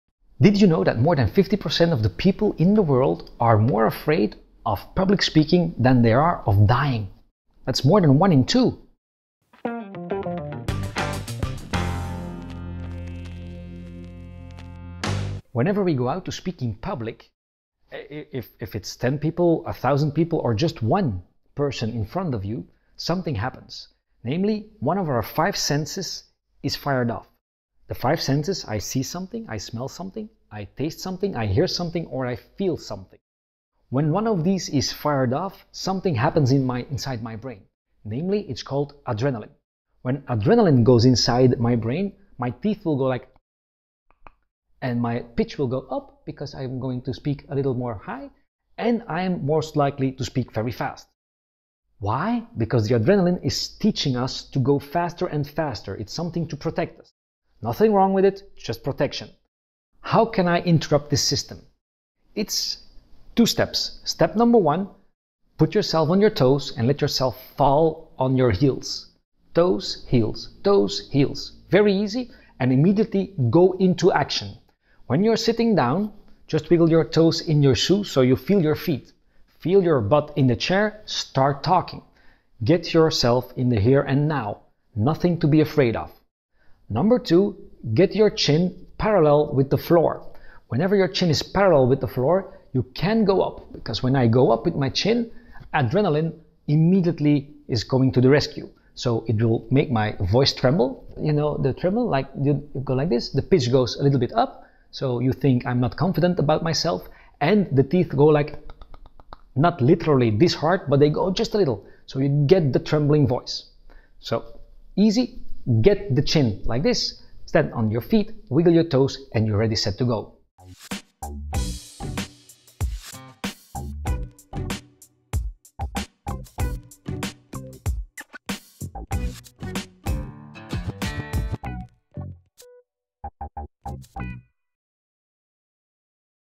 Public Speaking Anxiety - Tips To Overcome Your Fear
Many have experienced some level of anxiety when it comes to speaking in front of others. From a slightly elevated heart rate to clammy palms, public speaking can make even the most experienced speakers and presenters a bit nervous.

The fear of public speaking can become very intense and even debilitating. The anxiety can have an effect on our daily work or at school, or even events.

Despite the challenges that public speaking anxiety presents, there are strategies to manage its symptoms.

2 tips are explained in this video.